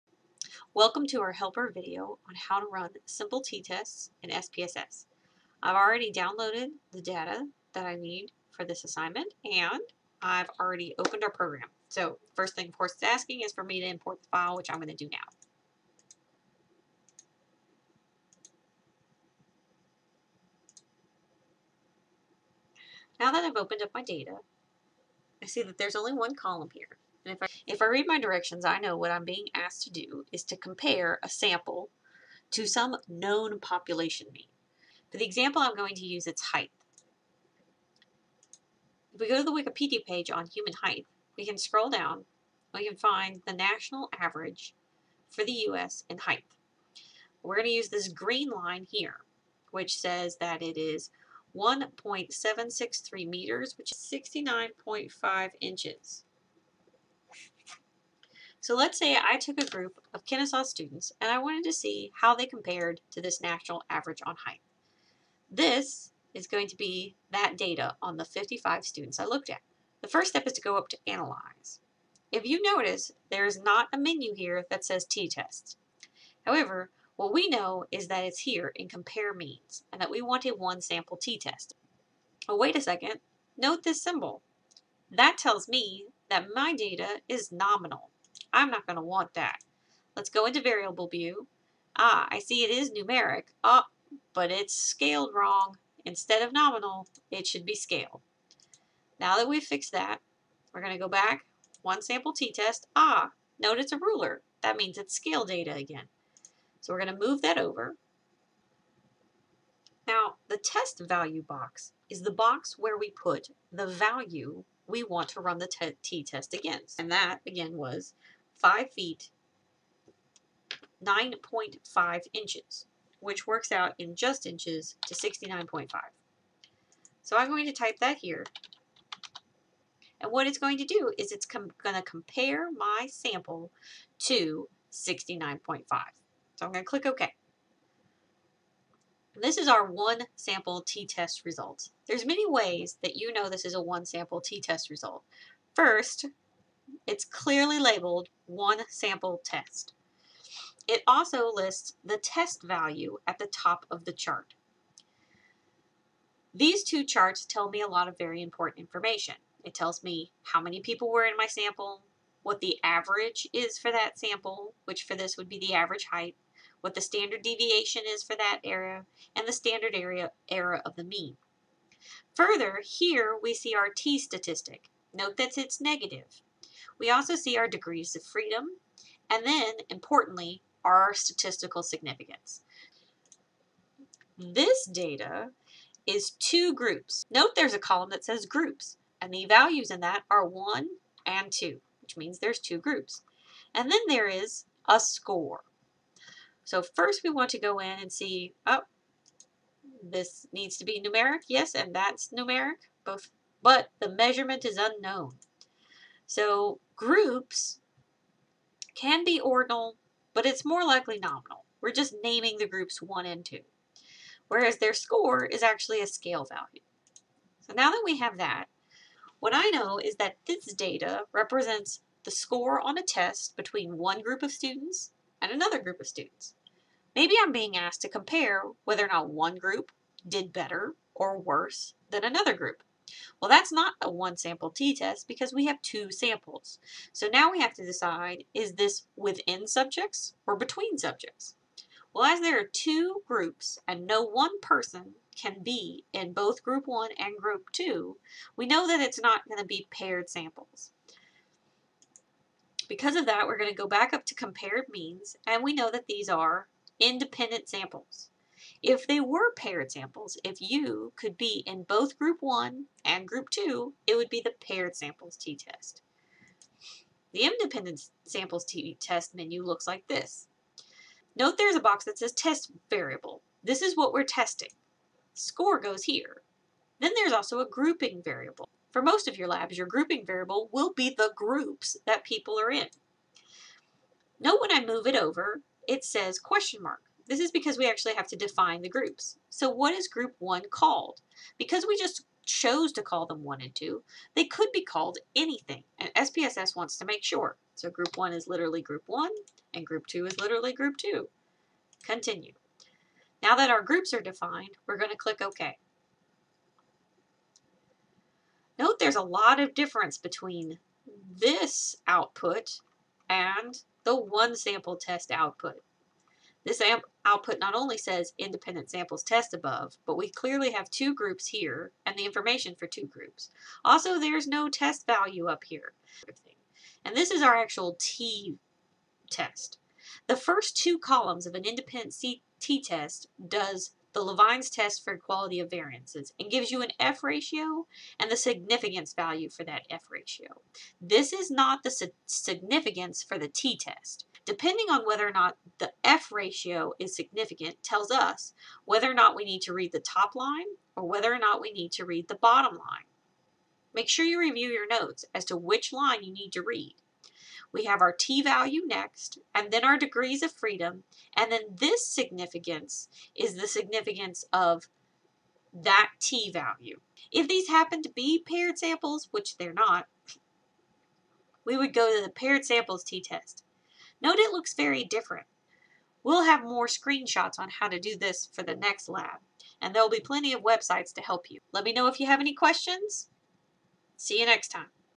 SPSS Helper Video - One Sample & Independent Sample t-tests
SPSS Helper Video for SPSS Labs: How to run a one sample t-test and an independent sample t-test